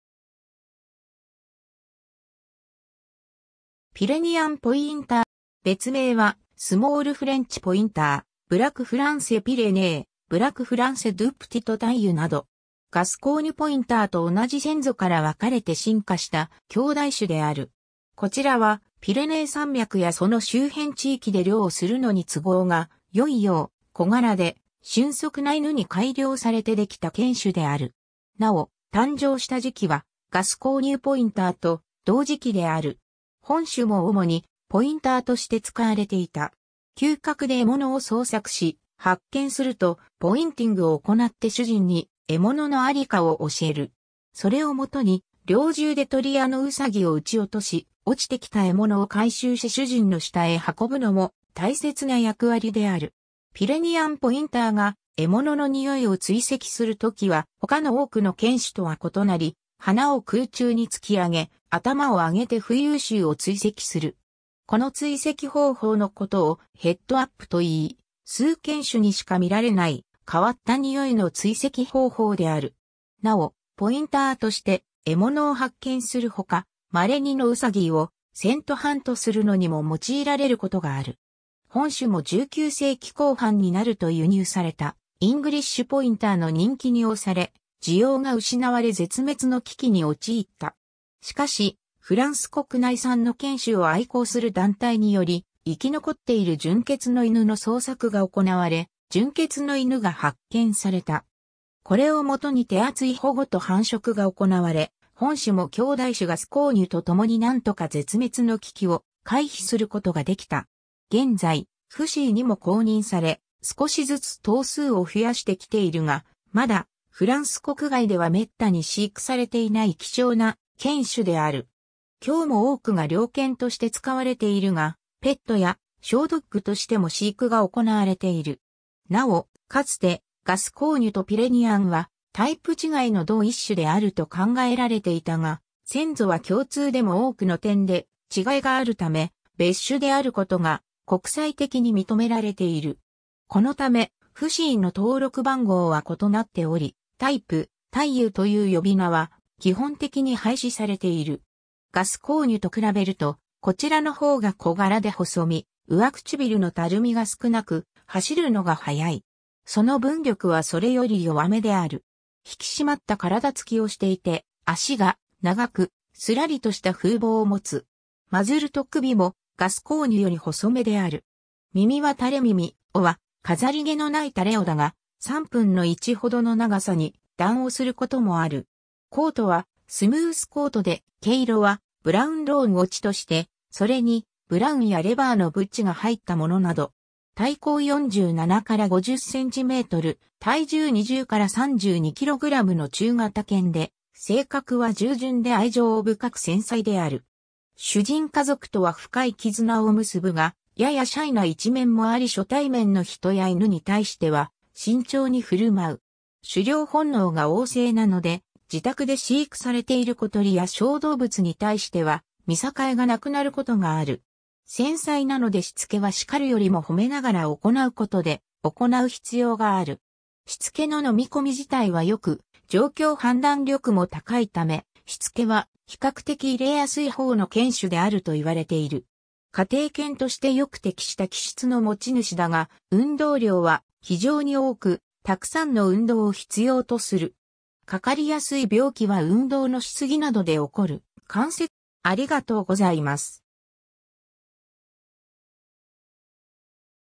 ピレニアン・ポインター
ポインター_(犬種)
ピレニアン・ポインター ピレニアン・ポインター（英：Pyrenean Pointer）は、フランスのピレネー山脈原産のポインター犬種のひとつである。
別名はスモール・フレンチ・ポインター（英：Small French Pointer）、ブラク・フランセ・ピレネー（英：Braque Français Pyrénées）、ブラク・フランセ・ドゥ・プティト・タイユ（英：Braque Français de Petite Taille）など。
 ガスコーニュ・ポインターと同じ先祖から分かれて進化した兄弟種である。
こちらはピレネー山脈やその周辺地域で猟をするのに都合がよいよう、小柄で俊足な犬に改良されて出来た犬種である。
尚、誕生した時期はガスコーニュ・ポインターと同時期である。
 本種も主にポインターとして使われていた。
嗅覚で獲物を捜索し、発見するとポインティングを行って主人に獲物のありかを教える。
それをもとに猟銃で鳥やノウサギを撃ち落し、落ちてきた獲物を回収し主人の下へ運ぶのも大切な役割である。
ピレニアン・ポインターが獲物の臭いを追跡するときは他の多くの犬種とは異なり、鼻を空中に突き上げ、頭を上げて浮遊臭を追跡する。
この追跡方法のことを「ヘッド・アップ」といい、数犬種にしか見られない変わった臭いの追跡方法である。
尚、ポインターとして獲物を発見する他、稀にノウサギをセントハント（嗅覚猟）するのにも用いられることがある。
 本種も19世紀後半になると輸入されたイングリッシュ・ポインターの人気におされ、需要が失われ絶滅の危機に陥った。
しかし、フランス国内産の犬種を愛好する団体により生き残っている純血の犬の捜索が行われ、純血の犬が発見された。
これを元に手厚い保護と繁殖が行われ、本種も兄弟種ガスコーニュとともに何とか絶滅の危機を回避することが出来た。
 現在FCIにも公認され、少しずつ頭数を増やしてきているが、まだフランス国外ではめったに飼育されていない希少な犬種である。
今日も多くが猟犬として使われているが、ペットやショードッグとしても飼育が行われている。
 尚、かつてガスコーニュとピレニアンはタイプ違いの同一種であると考えられていたが、先祖は共通でも多くの点で違いがあるため、別種であることが国際的に認められている。
このため、FCIの登録番号は異なっており、「タイプ」「タイユ」という呼び名は基本的に廃止されている。
 ガスコーニュと比べるとこちらのほうが小柄で細身、上唇のたるみが少なく、走るのが速い。
そのぶん力はそれより弱めである。
引き締まった体つきをしていて脚が長く、すらりとした風貌を持つ。
マズルと首もガスコーニュより細めである。
耳は垂れ耳、尾は飾り毛のない垂れ尾だが、3分の1ほどの長さに断尾することもある。
コートはスムースコートで、毛色はブラウン・ローンを地として、それにブラウンやレバーの斑が入ったものなど。
体高47〜50cm、体重20〜32kgの中型犬で、性格は従順で愛情深く繊細である。
主人家族とは深い絆を結ぶが、ややシャイな一面もあり初対面の人や犬に対しては慎重に振舞う。
狩猟本能が旺盛なので、自宅で飼育されている小鳥や小動物に対しては見境がなくなることがある。
繊細なのでしつけはしかるよりもほめながら行うことで行う必要がある。
しつけの飲み込み自体はよく、状況判断力も高いため、しつけは比較的入れやすいほうの犬種であるといわれている。
家庭犬としてよく適した気質の持ち主だが、運動量は非常に多く、沢山の運動を必要とする。
かかりやすい病気は運動のし過ぎなどでおこる関節疾患などがある。